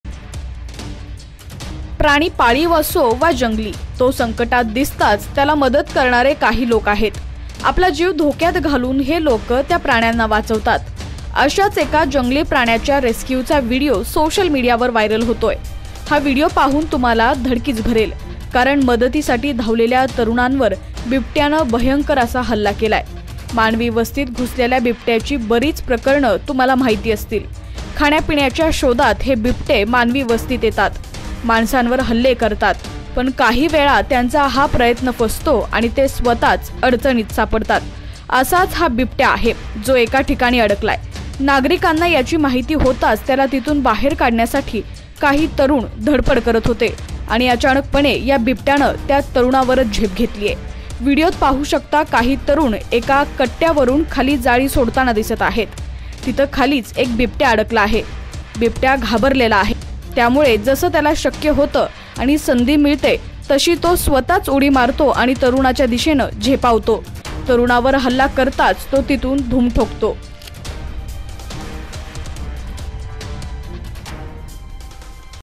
मदतीसाठी धावलेल्या तरुणांवर बिबट्याने केला हल्ला | Leopard attack video
प्राणी (Animal video) पाळीव असो वा जंगली, तो संकटात दिसताच त्याला मदत करणारे काही लोक आहेत. आपला जीव धोक्यात घालून हे लोक त्या प्राण्यांना वाचवतात. अशाच एका जंगली प्राण्याच्या रेस्क्यूचा व्हिडीओ सोशल मीडयावर व्हायरल होतो आहे. हा व्हिडीओ पाहून तुम्हाला धडकीच भरेल. कारण मदतीसाठी धावलेल्या तरुणांवर बिबट्याने भयंकर (Leopard attack video) असा हल्ला केला आहे (Leopard video).